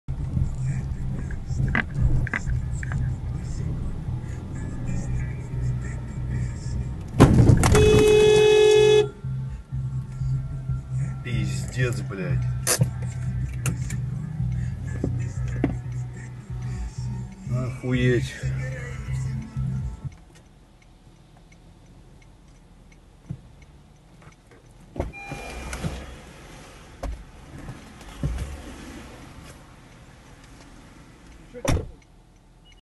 ДТП на пл. Мужества. Джамшут, показывая видео со своего регистратора говорит, чуть чуть не считается
Ссылка на видео с регистратора Джамшута
Юридическая помощь в ГИБДД, в Страховых 😎
Санкт-Петербург
АВТОЮРИСТ